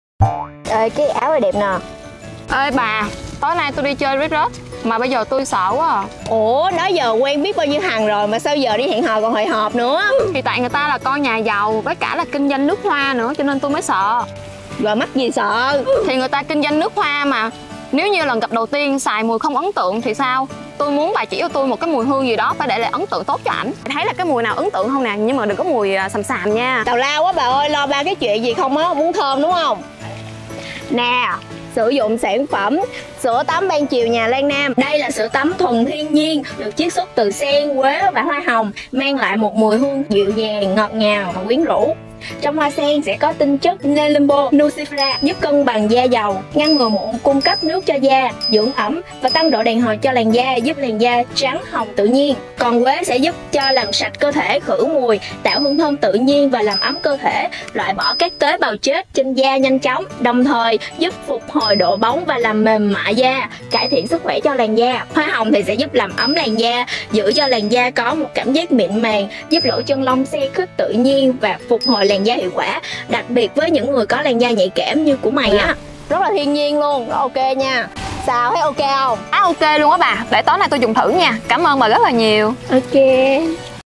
Bí kíp cho các nàng có cuộc hẹn hò đẹp
Nếu bạn đang lo lắng, đau đầu khi lựa chọn mùi hương hẹn hò. Đừng lo Sữa tắm thiên nhiên nhà L'AnnAm sẽ giải quyết giúp bạn. Với hương thơm từ các thành phần thiên nhiên, mùi thơm ngọt ngào, dễ chịu. Sữa tắm thiên nhiên L'AnnAm không chỉ giảm bớt mùi cơ thể, lưu hương thơm ngọt ngào, mà còn kháng khuẩn và giúp da mịn màng, trắng hồng tự nhiên